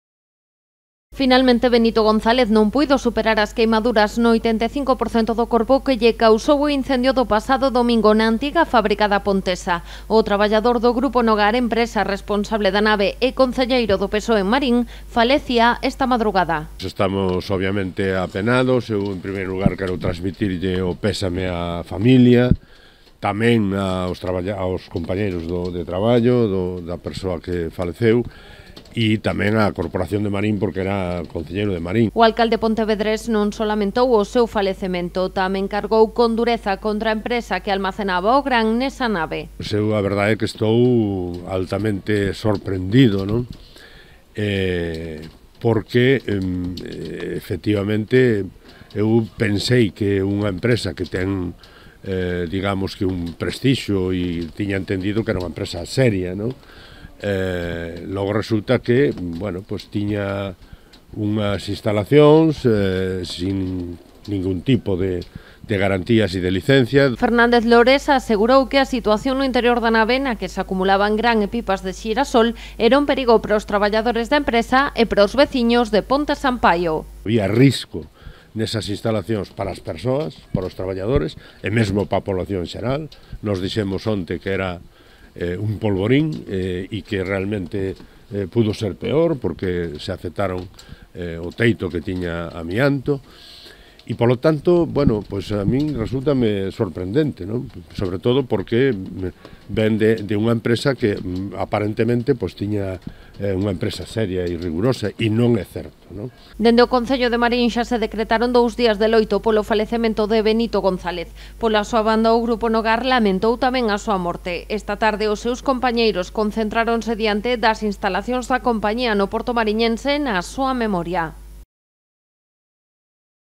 Fernández Lores, sobre o Grupo Nogar: ‘Pensei que era unha empresa seria’
Esta madrugada falecía o traballador que resultara ferido de gravidade no incendio da antiga fábrica da Pontesa, na parroquia pontevedresa de Ponte Sampaio. O home, veciño de Marín, presentaba queimaduras no 85% do seu corpo. Tras coñecer a triste noticia, dende o concello de Pontevedra cargaron contra o Grupo Nogar, responsable da nave que ardeu. O alcalde capitalino recordou que carecía de licenza de actividade e de medidas de seguridade.